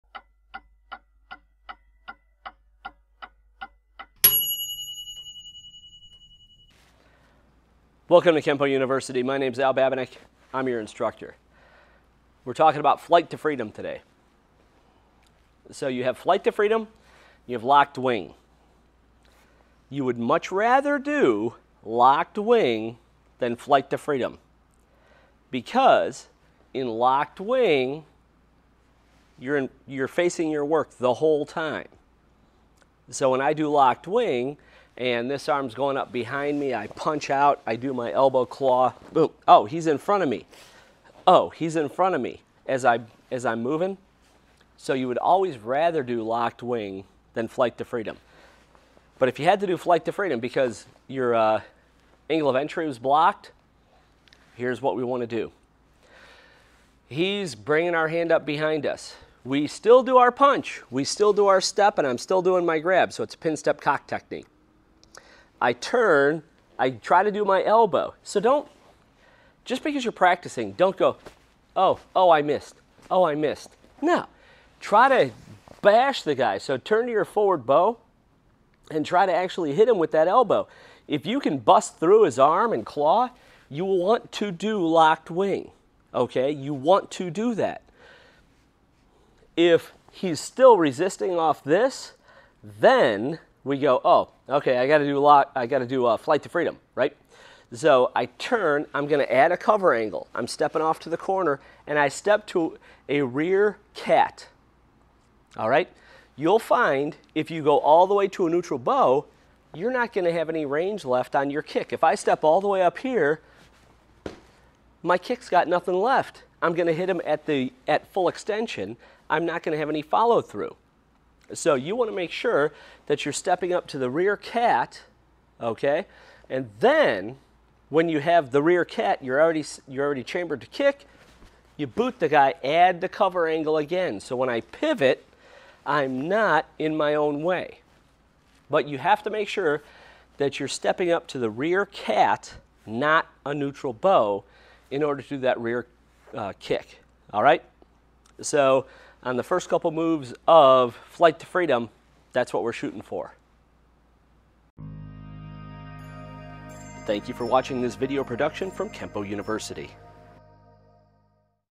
Flight to Freedom 1st move
All about the basics of Flight To Freedom. The Pin, Step, Chamber. The rear cat. The cover angles.